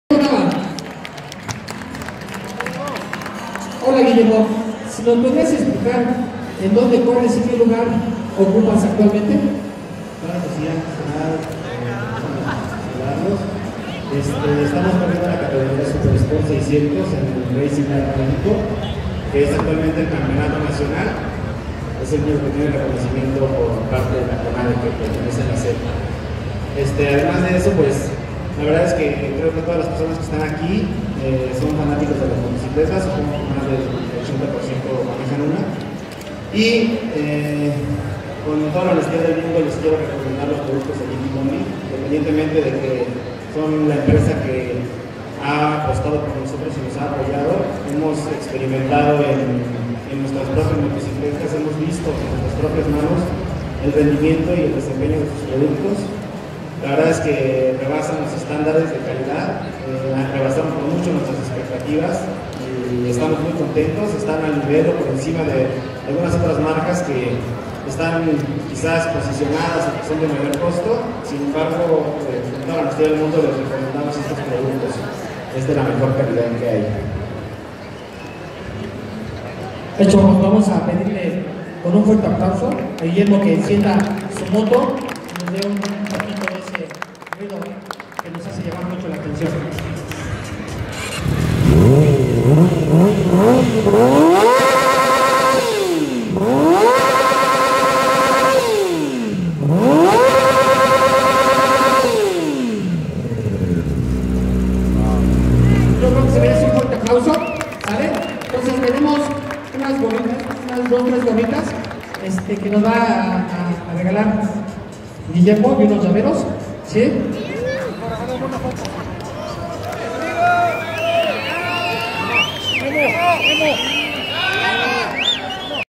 Piloto Guillermo Italika Racing Super Sports 600cc Expo Moto 2017
Piloto Guillermo Italika Super Sports 600cc  Expo Moto 2017  Como suena la moto italika Racing 600 cc